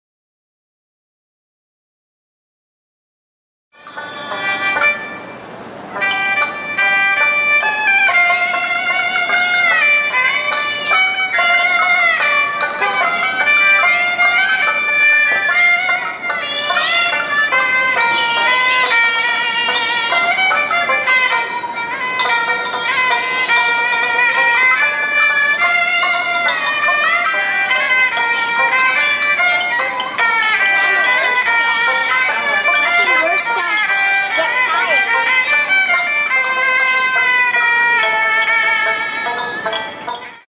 los dragones
En directo desde China Town de San Francisco